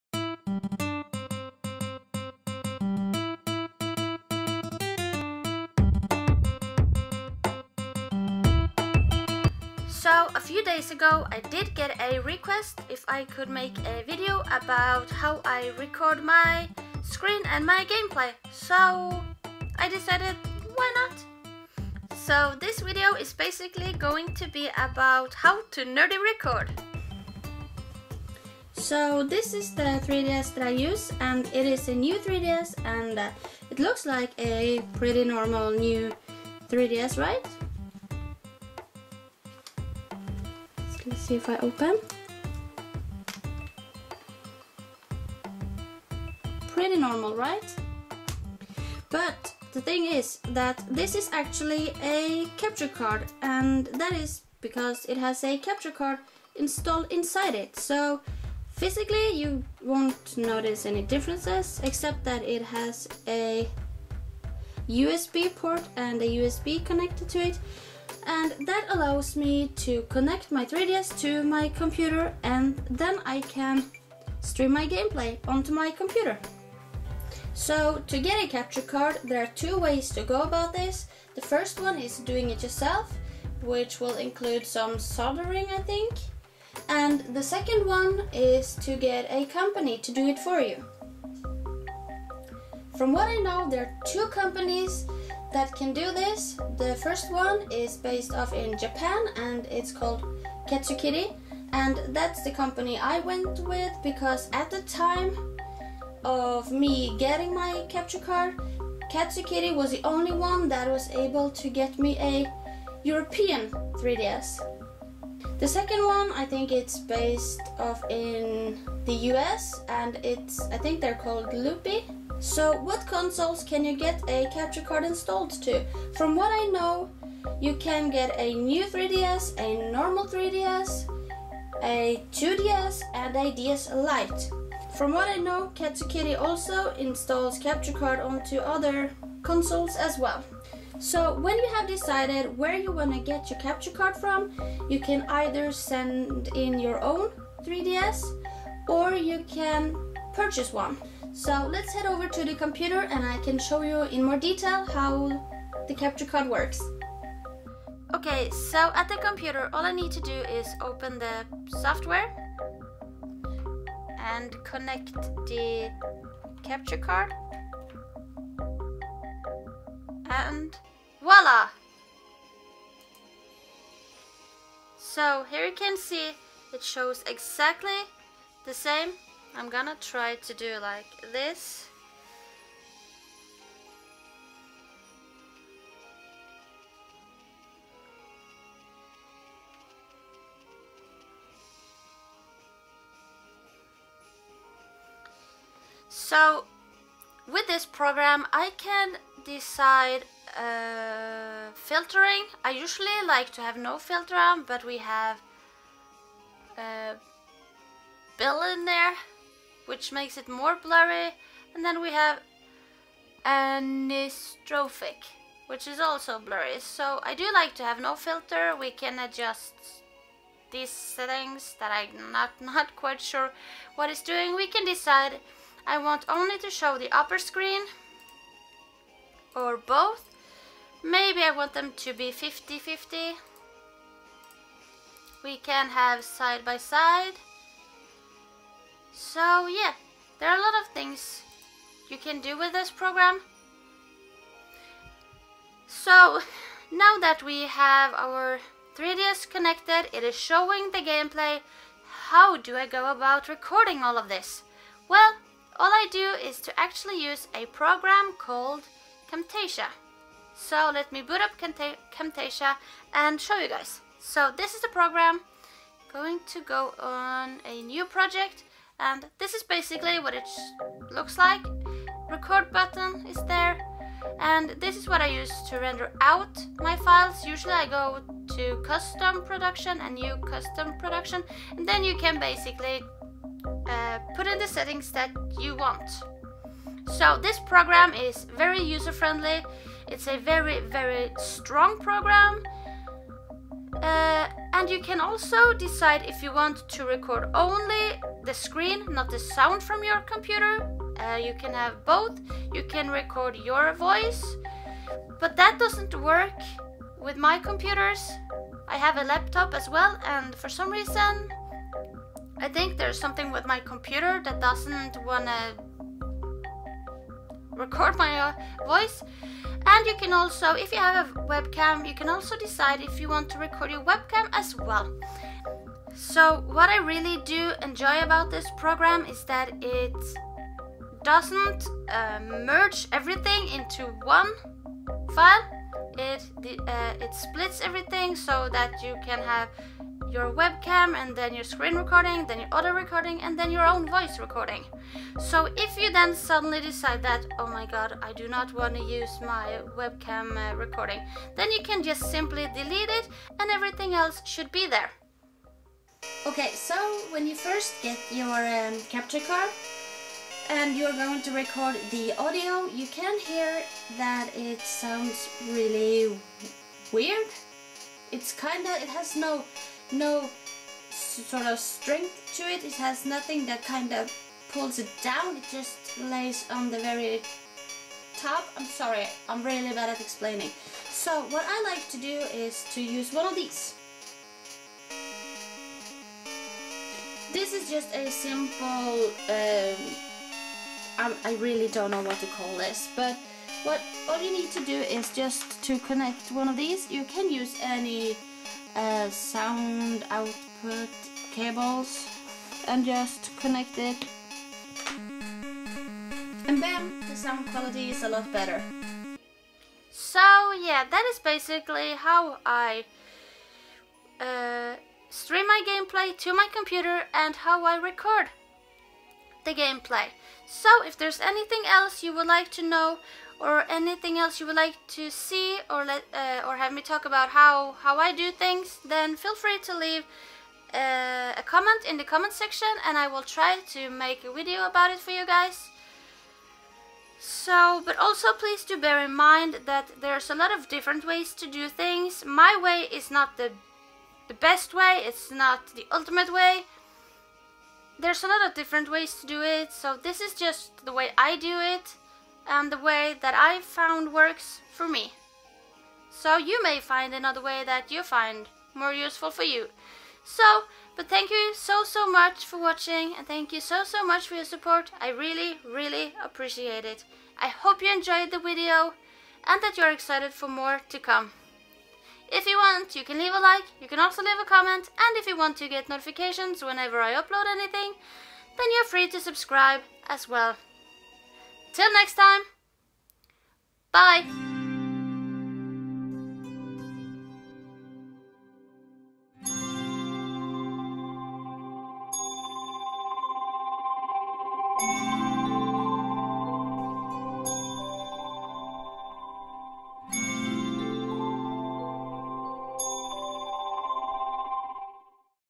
HOW TO RECORD 3DS GAMEPLAY AND PC SCREEN - HOW TO NERDY
So like I said in the video I did get a request  about making a video  showing how I recrod my 3ds and my computer screen. If there is anything you want med to show how I do like, layout and thumb making, or in general movi editing, let me know in the comment section =3 if You guys want I can make a how to nerdy series about how i do my stuff I can uploade them once i week, mabye on friday`s ?=) let me know;)